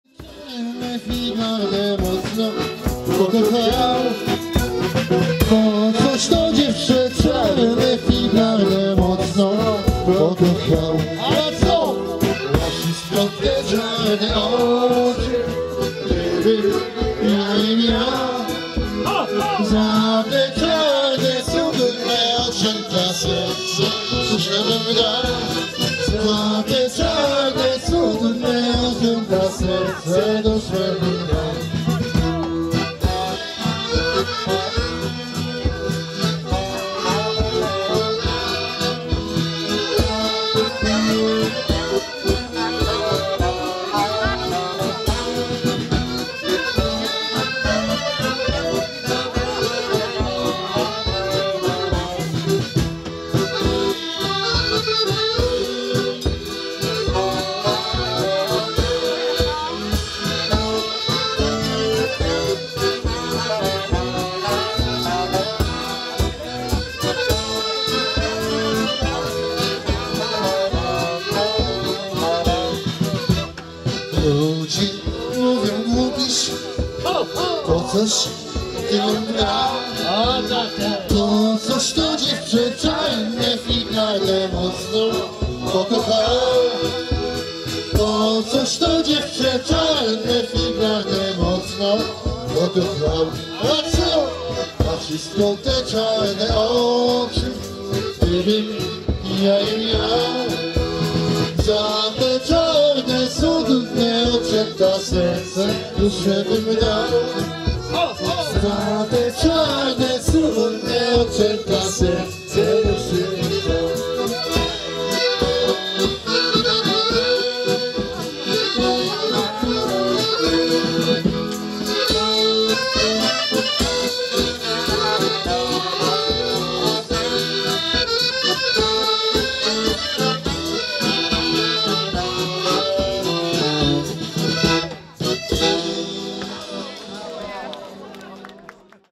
Elegancka wymiana PAR ~Organizator też znalazł czas aby zatańczyć !
Kapela Tomasza Stachury na Ludowej Zabawie w Ostałowie